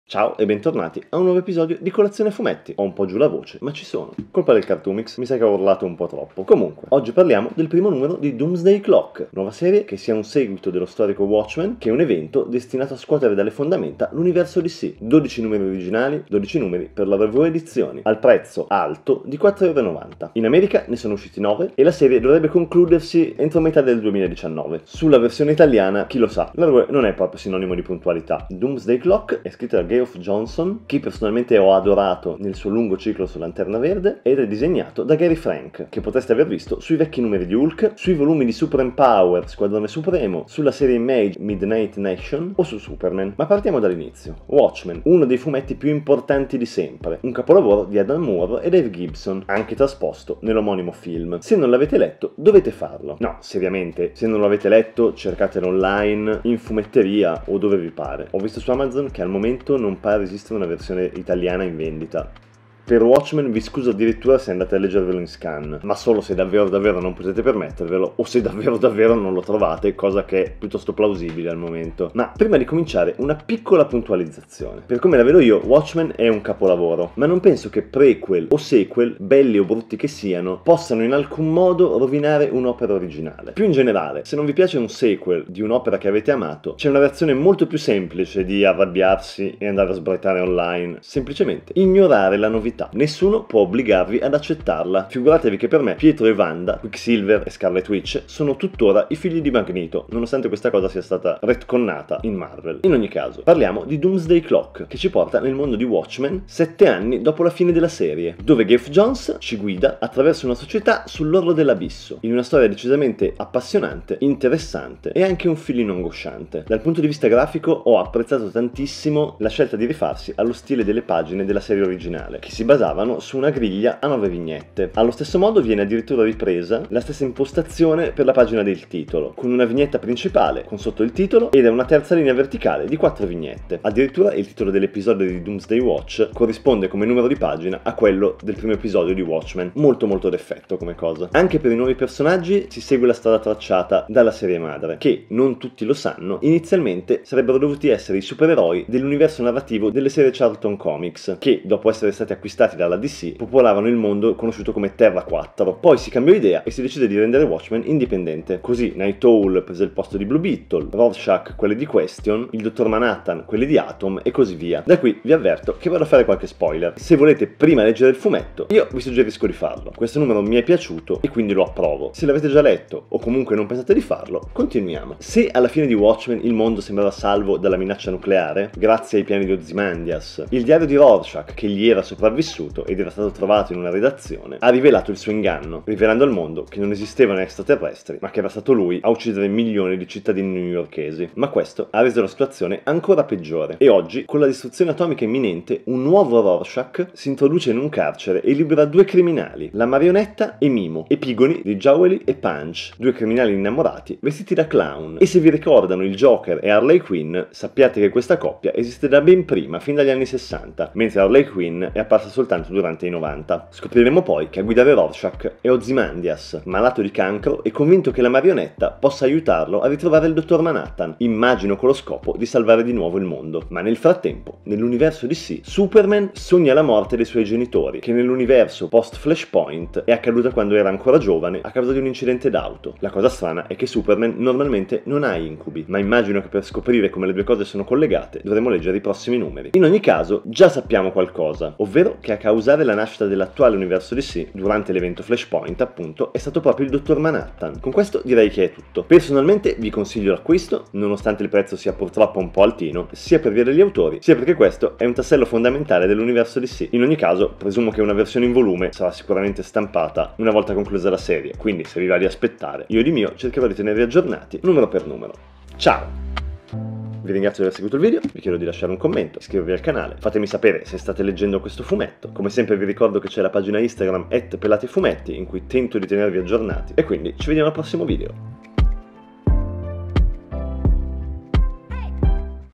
Doomsday Clock #1: il seguito di Watchmen
Oggi parliamo del primo numero di Doomsday Clock, seguito ufficiale di Watchmen, e niovo grande evendo in casa dc comics!